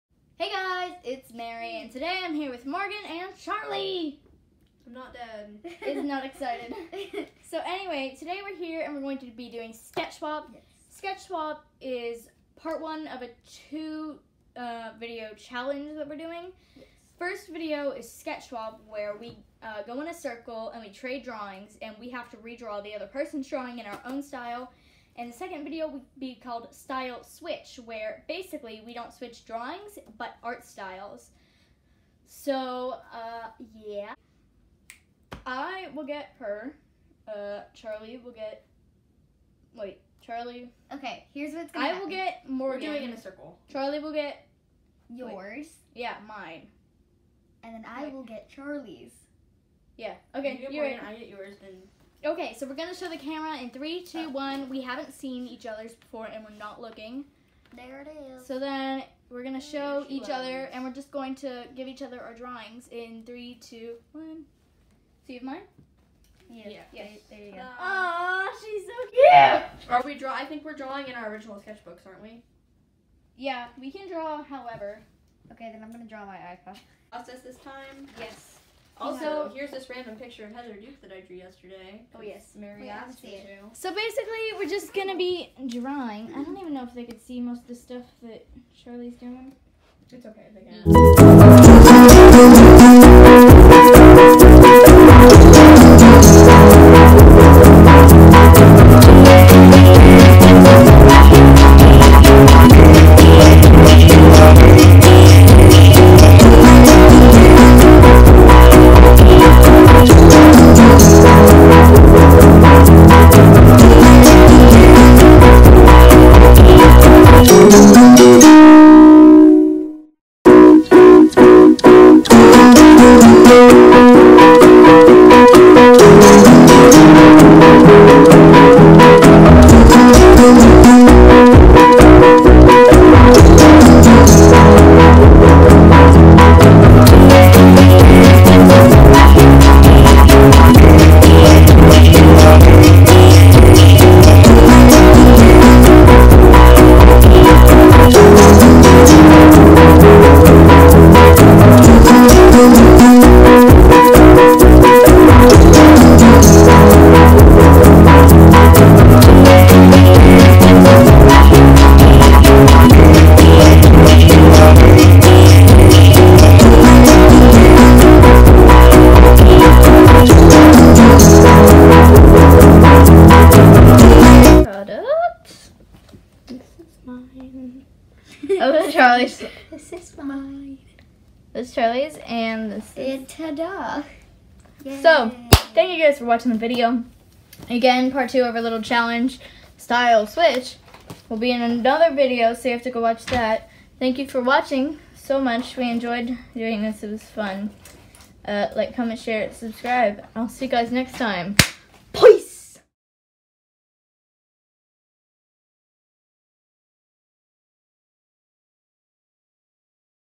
Sketch Swap No. 1
Hello! This is a fun art challenge I did with Morgan and fjoriku!